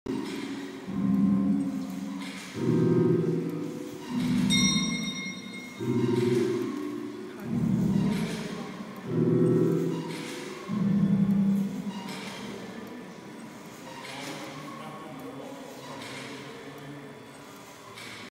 Short clip of installation at Belgium Pavilion at the 2019 Venice Biennale.
Watch closely. Each figure is making or creating something... pottery, music, painting. The taskmaster rings the bell to maintain order.